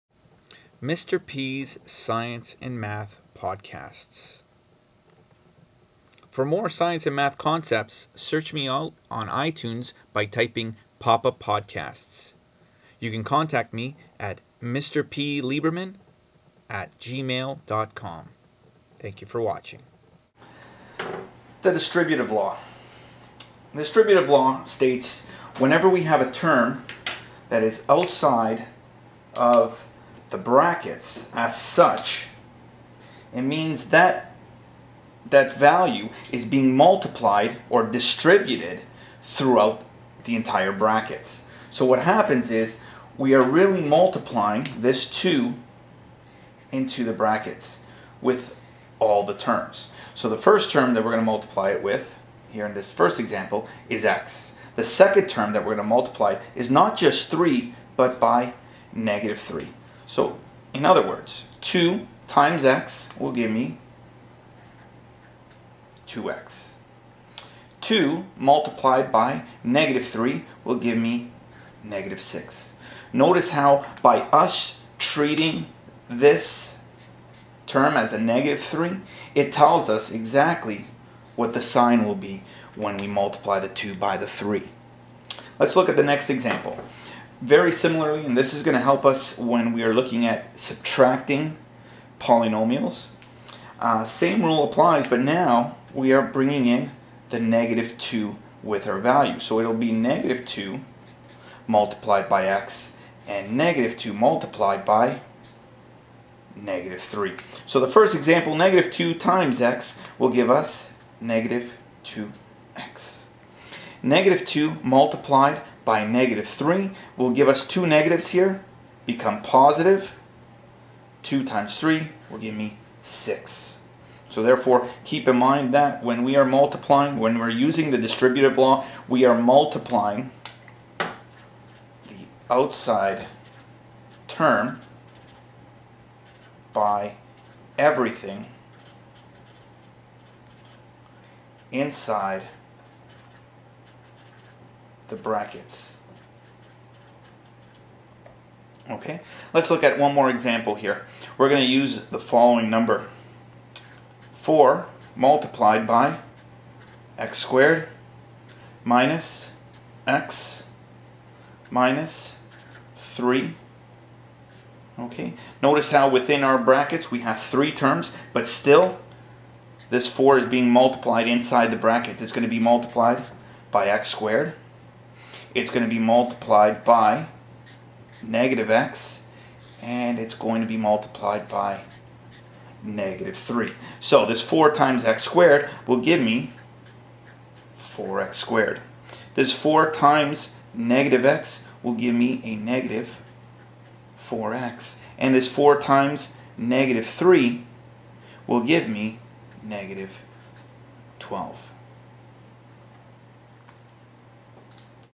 Distributive Law
The following lesson looks at the Distributive Law. Download for FREE and watch on Full Screen using iTunes, by typing the word "Papapodcasts" in the search window.